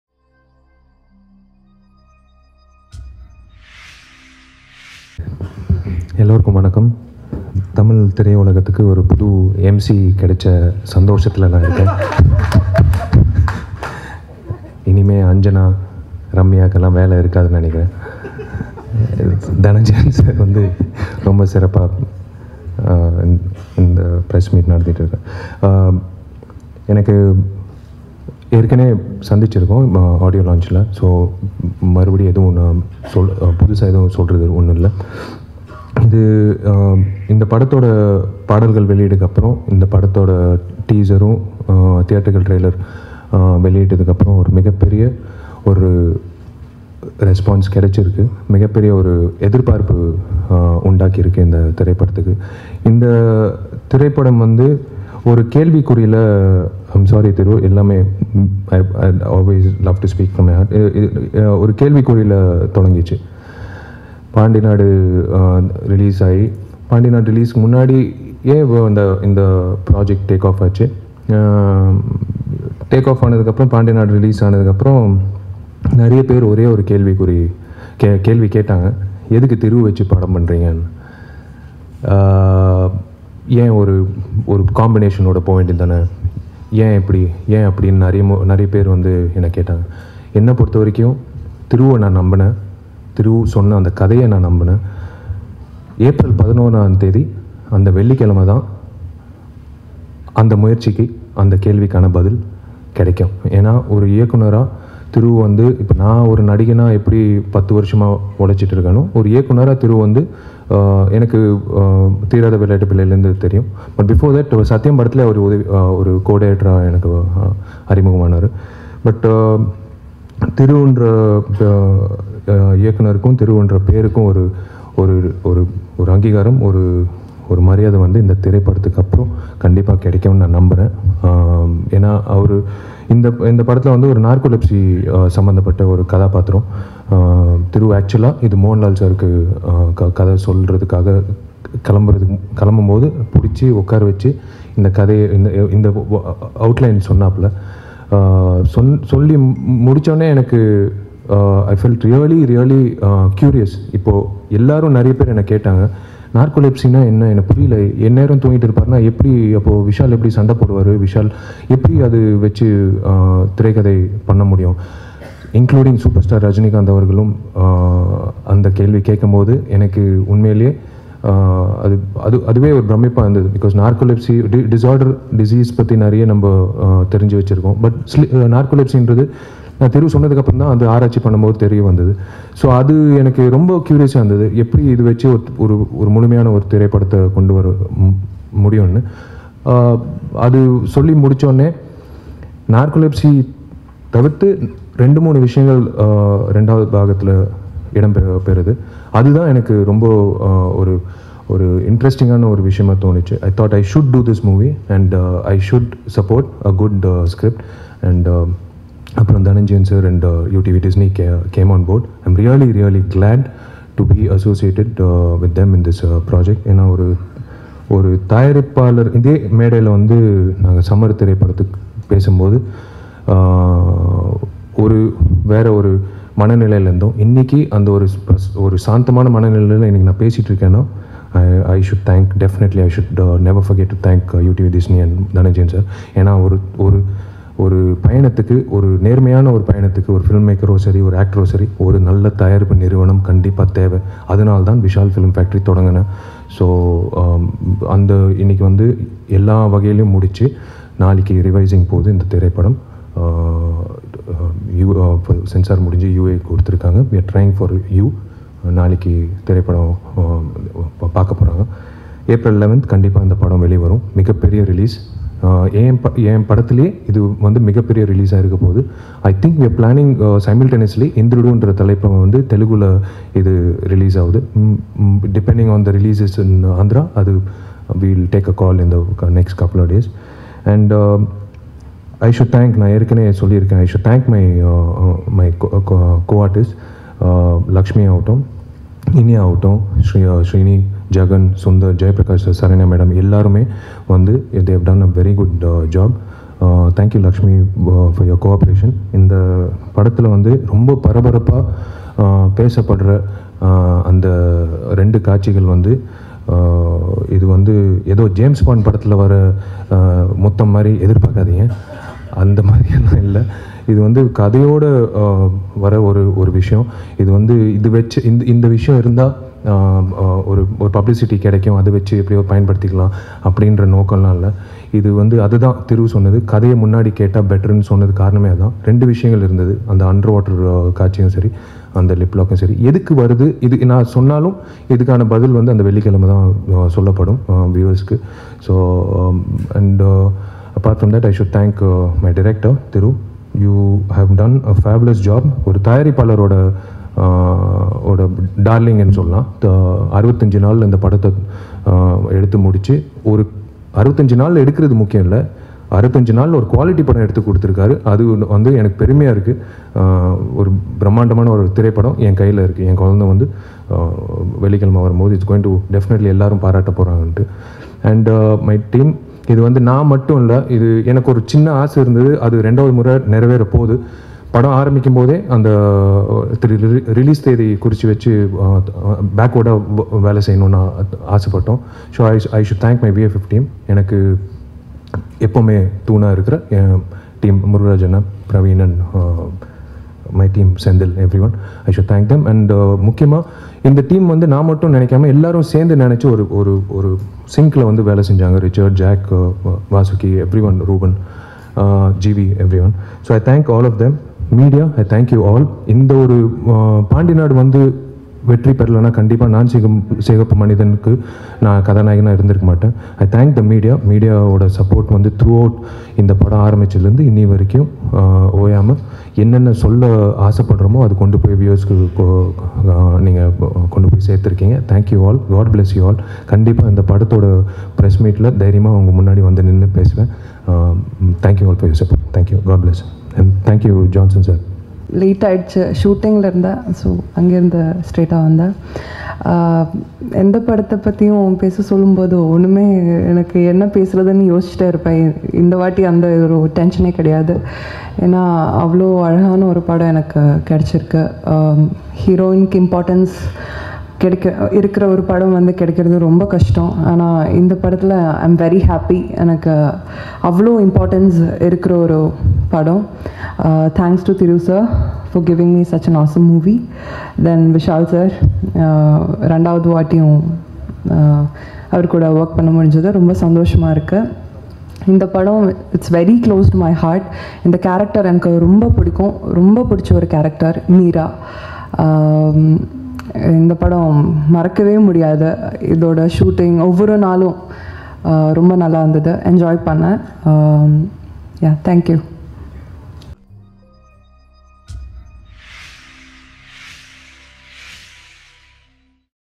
Vishal and Lakshmi Menon at Naan Sigappu Manithan Press Meet
Naan Sigappu Manithan Movie Press Meet held on 02nd Apr 2014 at Prasad Lap, Chennai. Vishal, Lakshmi Menon, Iniya, Thiru, Vasuki Bhaskar, G. Dhananjayan, Anthony L. Ruben, Richard M. Nathan and others took part in the event.